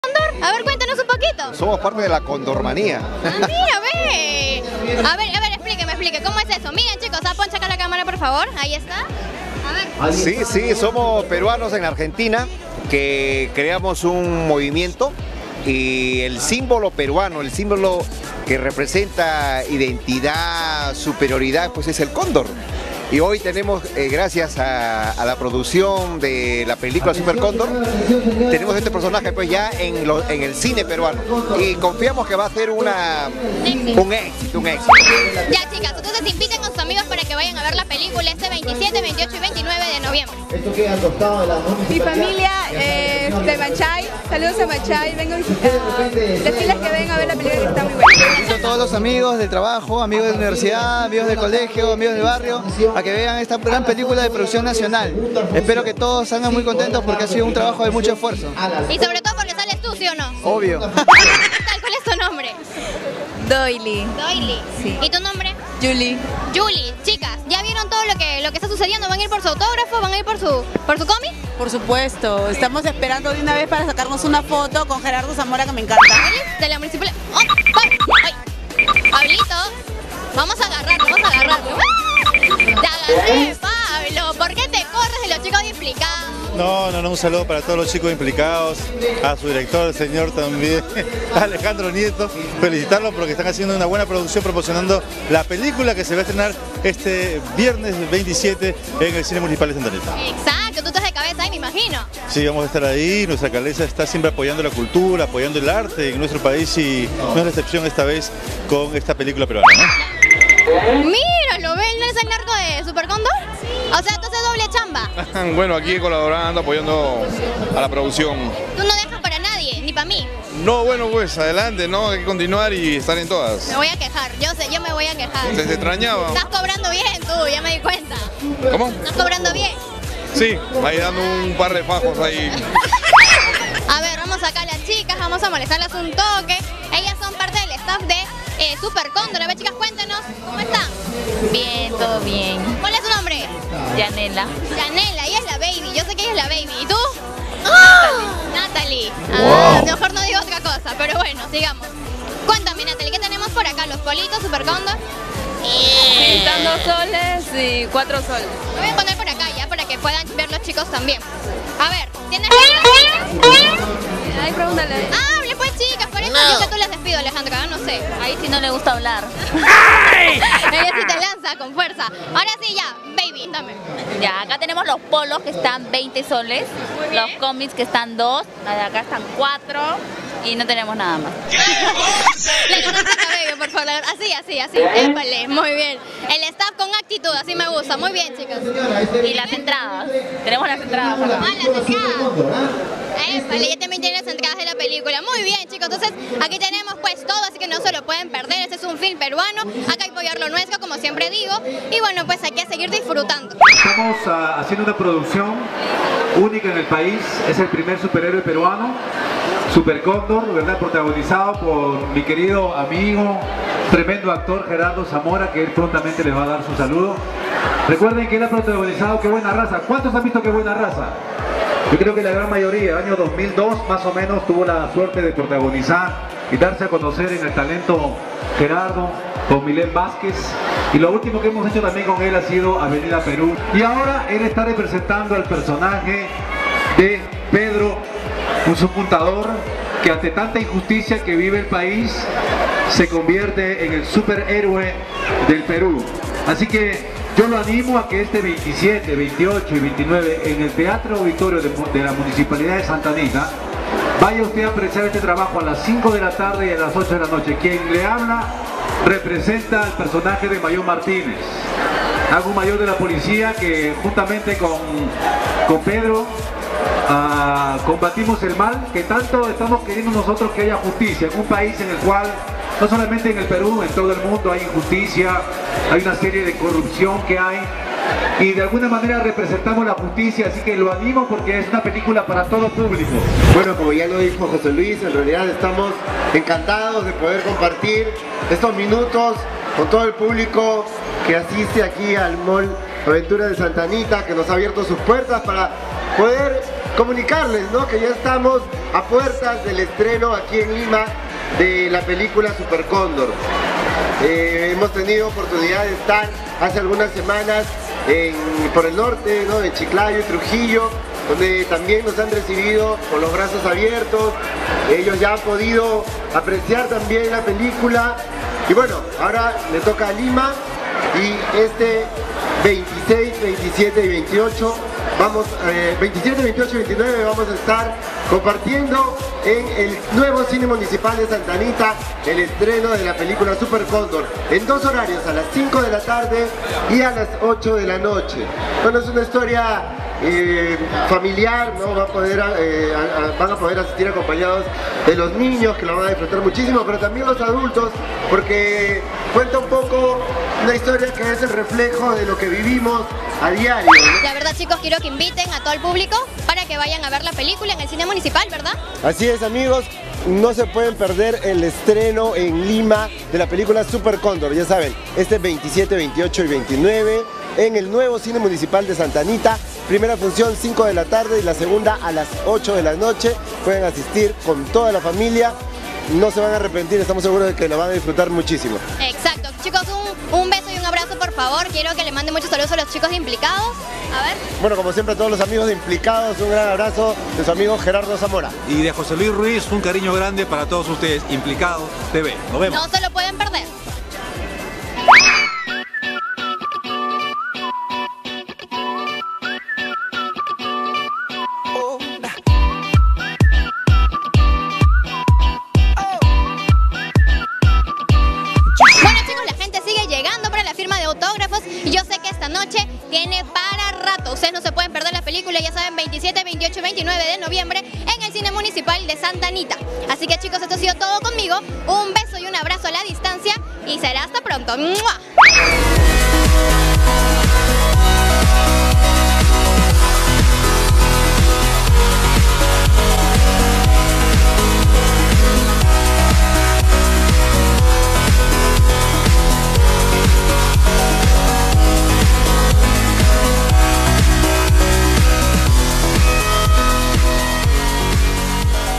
SÚPER CÓNDOR: Los protagonistas de la película y la Cóndormania
Ver y escuchar las palabras de los protagonistas:
* Minuto 00:01 al 00:35 -  Fernando Vargas Quiroz (fans de Cóndormania)
* Minuto 03:24 al 04:41 - Jose Luis Ruiz (actor, Mayor PNP Martínez )
* Minuto 04:42 al 05:53 - Gerardo Zamora (actor, Súper Cóndor --- )